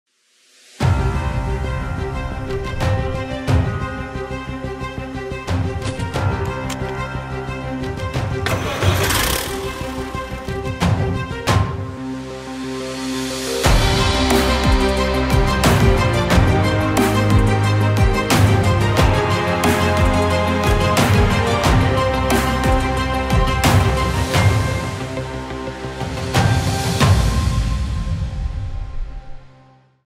Cat® 216B3 Skid Steer Loader
Built for tough work, the Caterpillar® Skid Steer Loaders deliver Cat reliability, durability, and efficient operation. Watch the latest model of Cat® 216B3 Skid Steer Loader in action.